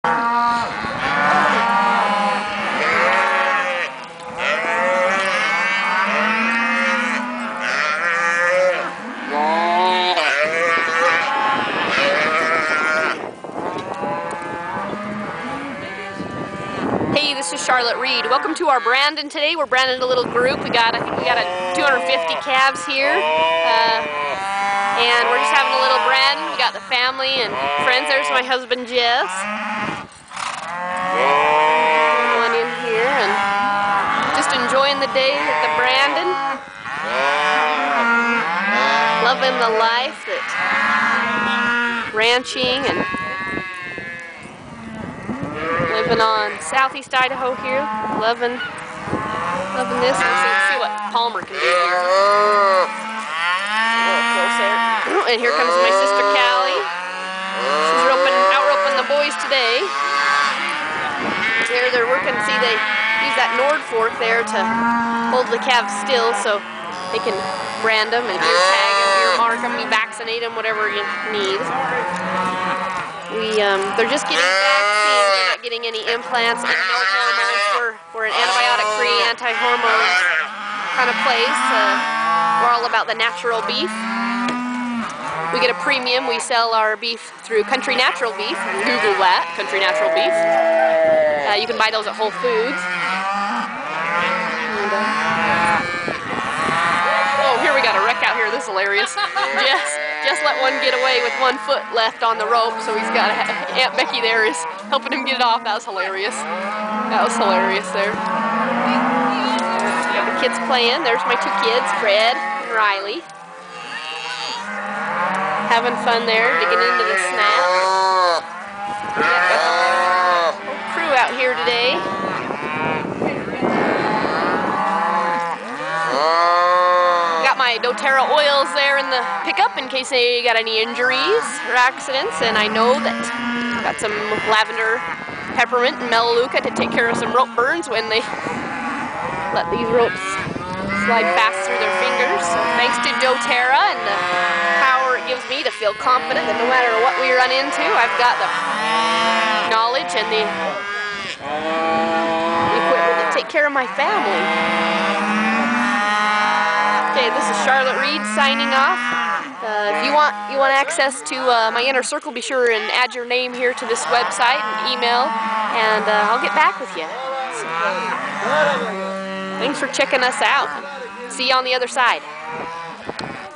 Branding Cattle on Reid Ranch
Branding day in Southeast Idaho. See more updated videos at our ReidRanch channel, or at the This video shows the ranch crew branding cattle several years ago on Reid Ranch.